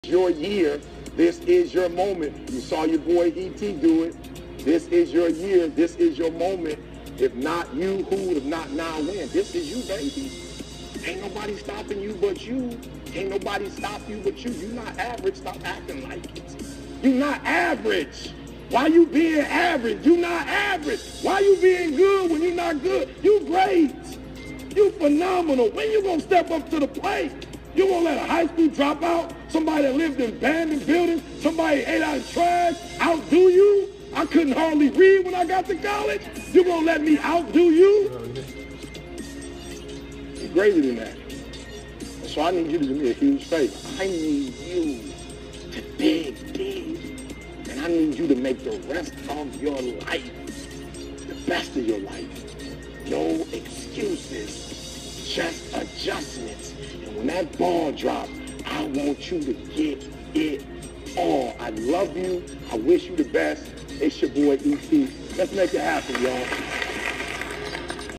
Eric Thomas- "You're Not Average, You're Great"
Motivational speaker Eric Thomas gives an inspirational message.